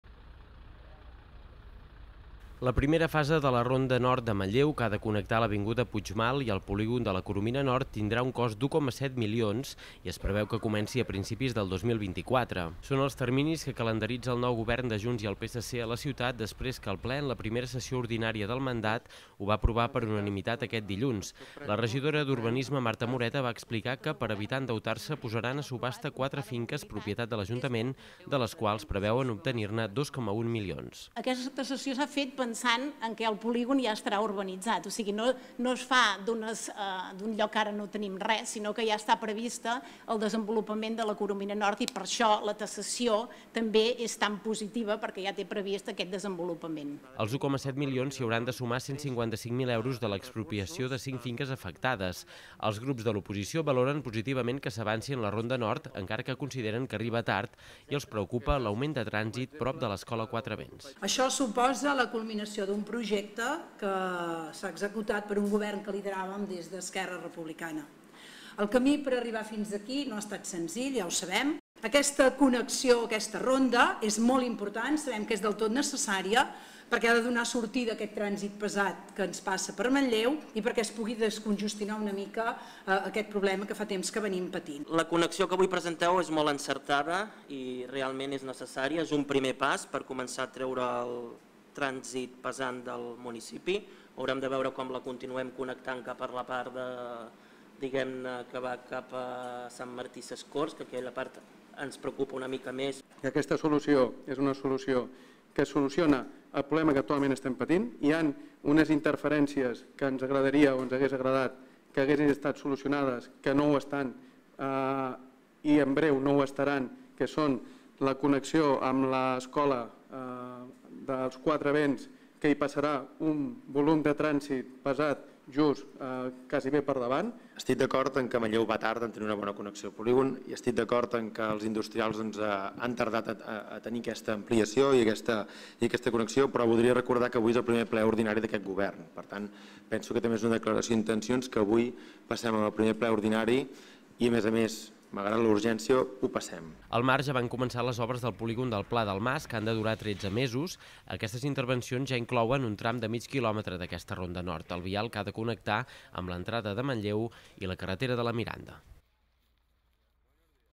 Les obres de la ronda nord de Manlleu arrencaran a principis de 2024
El ple de Manlleu va aprovar per unanimitat aquest dilluns el projecte d'urbanització de la ronda nord, entre l'avinguda Puigmal i el polígon de la Coromina. Es preveu que la primera fase s'executi a partir del mes de gener i que tingui un cost aproximat d'1,7 milions d'euros. El consistori, pilotat ara per Junts per Catalunya i el PSC, posarà a subhasta quatre finques de l'Ajuntament que sortiran a un preu de 2,1 milions d'euros.

INFORMA: Guillem Rico